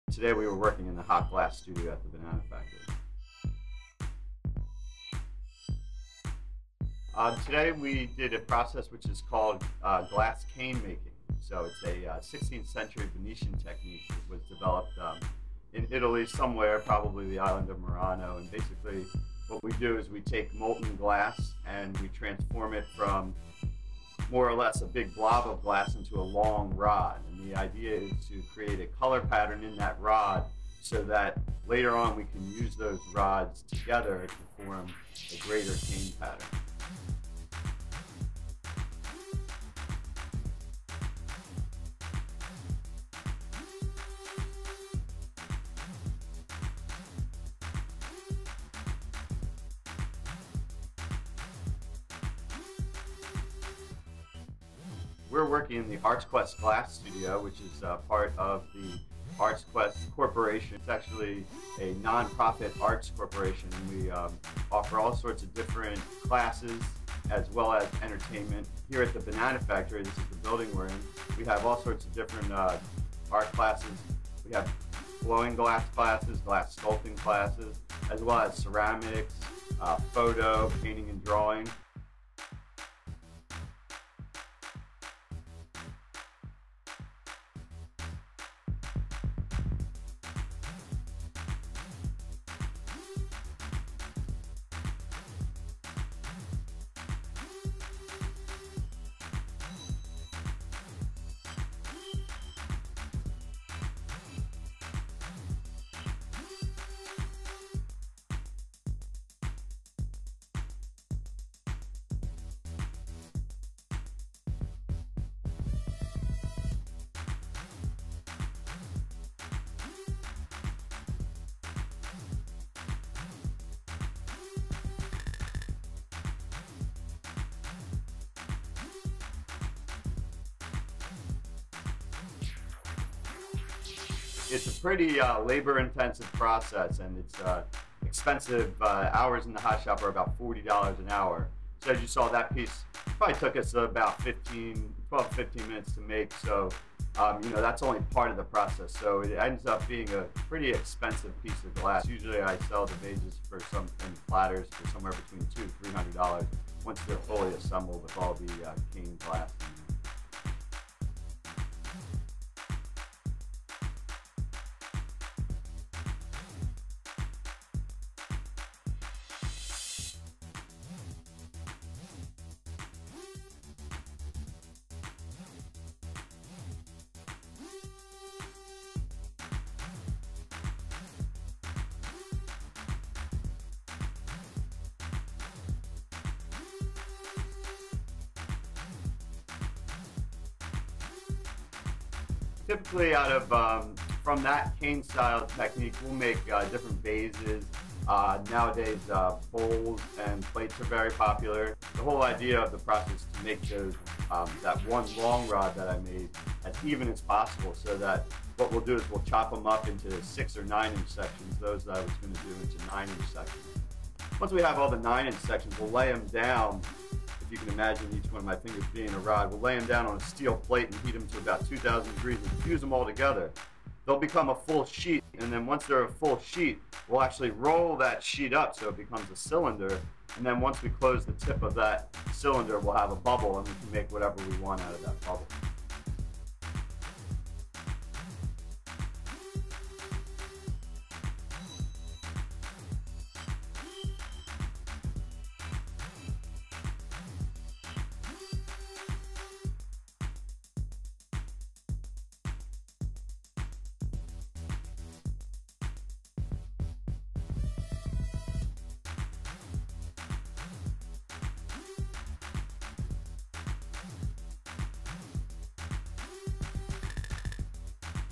Creating With Glass: Glass Blowing at the Banana Factory
An inside look at Glassblowing at the ArtsQuest Glass Studio in Bethlehem, PA.

This video was primarily filmed using Google Glass.